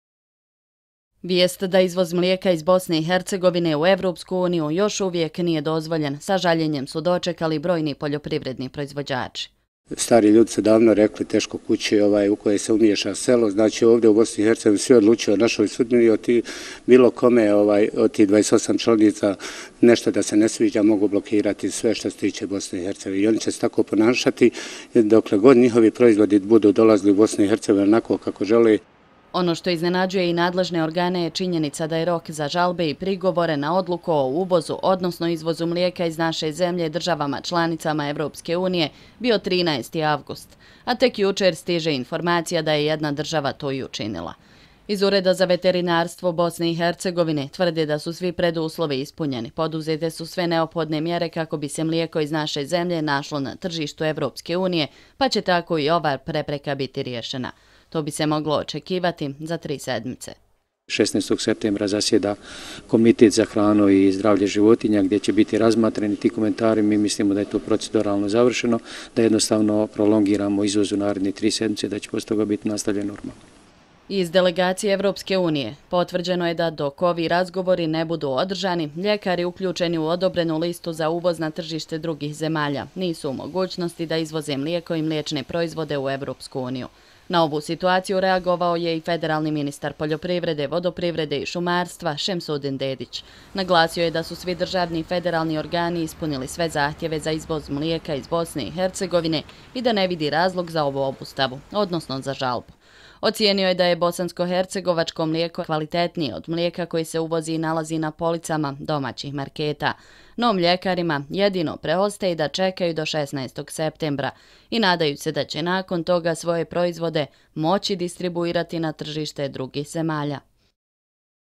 Nikšić o obustavi izvoza mlijeka: „Teško kući u koju se umiješa selo“ - 27.08.2015.
Iako su poljoprivrednici sa nestrpljenjem iščekivali taj dan kada će moći izvoziti mlijeko i mliječne prerađevine iz BIH u zemlje Evropske unije, na putu im se našla još jedna prepreka. Izvoz mlijeka iz Bosne i Hercegovine još nije dozvoljen, nakon što je jedna od članica zemalja Evropske unije uložila žalbu. Nadležni očekuju da će problem biti riješen u narednih nekoliko sedmica.